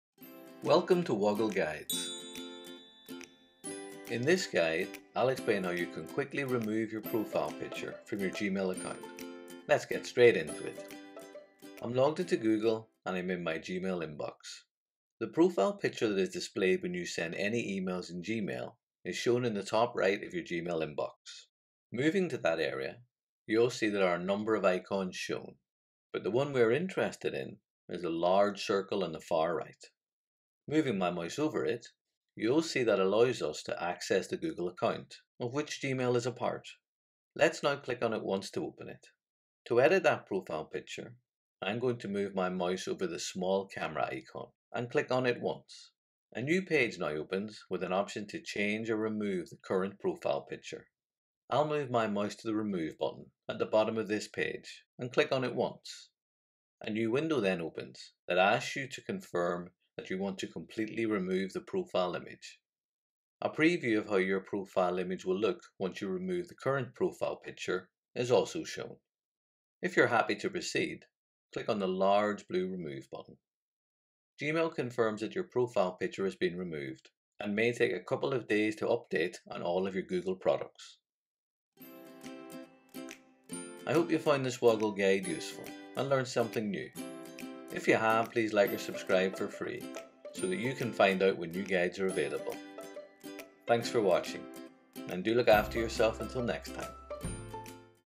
How to remove a Gmail profile picture
In this guide, I’ll explain how you can quickly remove a profile picture on your Gmail email account.

🕔  Key Moments
00:00 | Introduction
00:12 | How to remove a profile picture from your Gmail account
01:23 | Wrap up

🔎  Useful links
See more info on how to change your profile picture in Gmail in this article from the Google Support Center - ‘Change your Gmail profile picture’ -